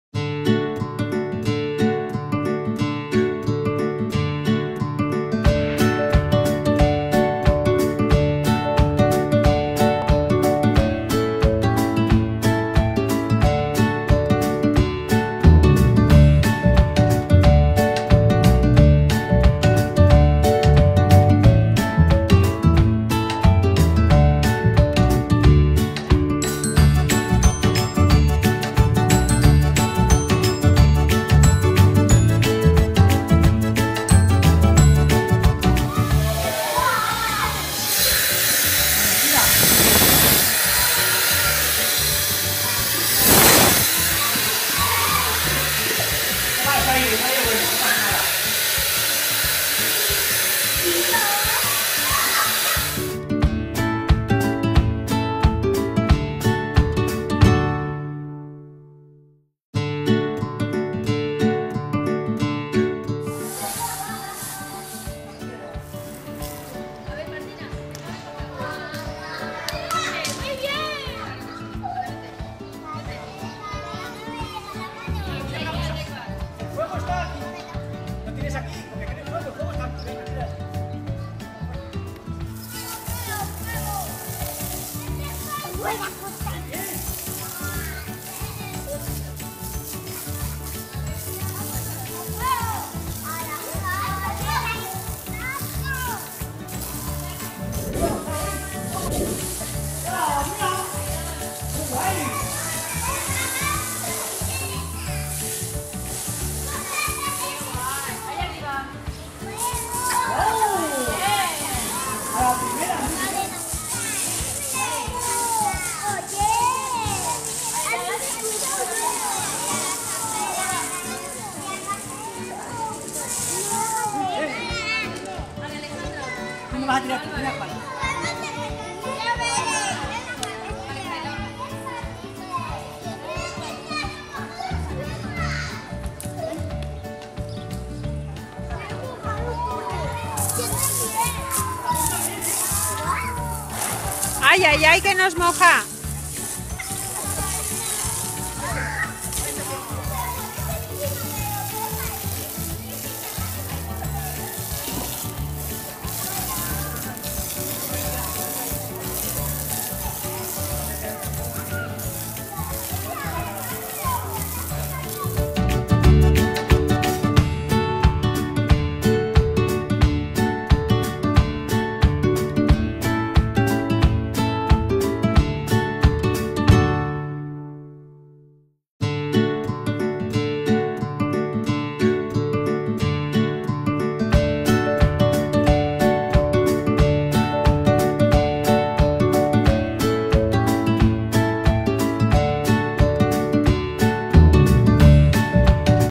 Infantil 5 años. Aprendemos a ser bomberos.
El lunes tuvimos una sorpresa ...nos visitó un bombero. ¡Cuántas cosas nos enseñó!
Aprendimos qué hacer en caso de cualquier emergencia, cuál es el equipaje que utilizan en cada caso, vimos un video muy educativo, en el patio y con una manguera especial apagamos el fuego de un edificio en llamas y para terminar hicimos un simulacro de incendio dentro de la clase. Toda una ¡¡¡¡¡¡AVENTURA! Y para que no se nos olvide este día tan fantástico, nuestro bombero, nos regaló un casco rojo con nuestro nombre...GRACIAS JOSE.